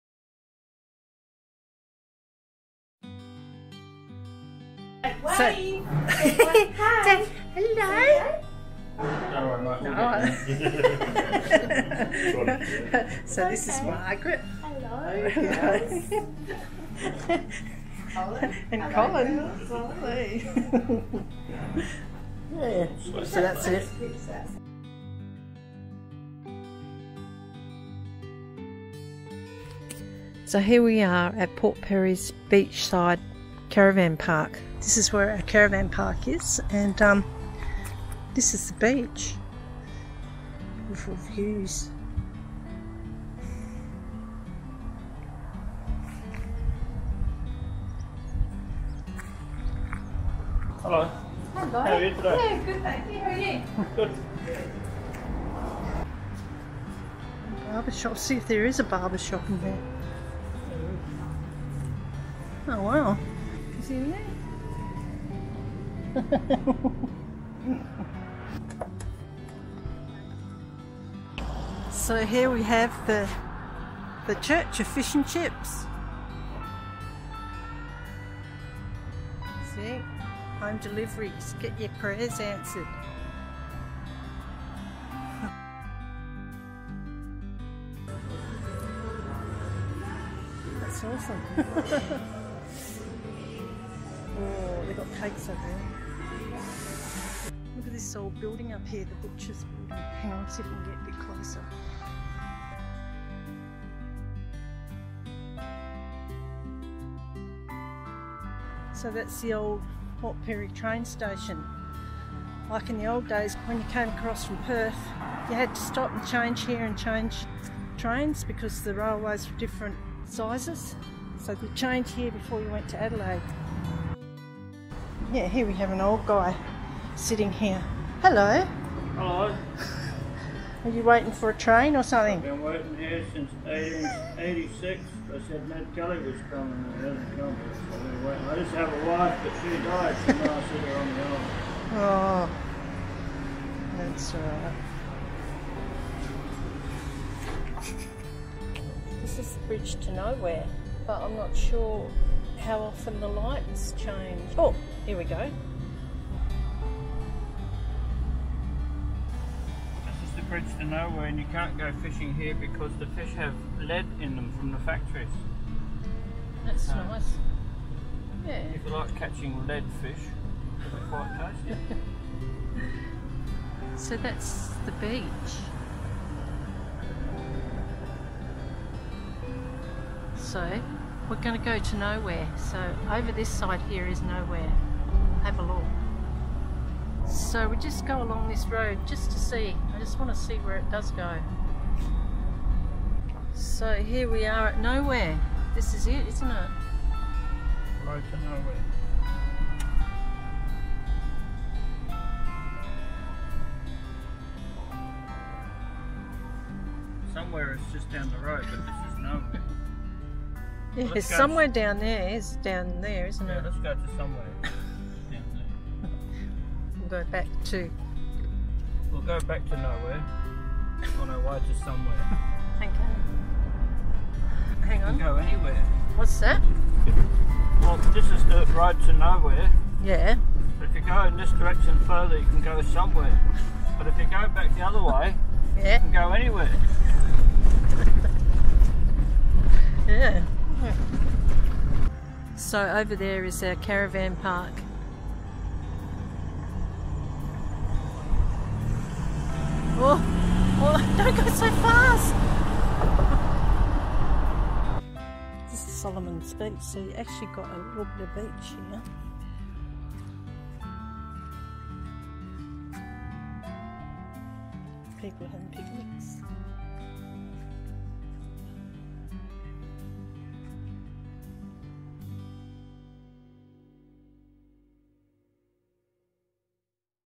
PORT PIRIE WALK AROUND
We called in to see my cousin Margaret and her family on the way, lovely to see them.  Then a short tour of Port Pirie the highlight being the bridge to nowhere,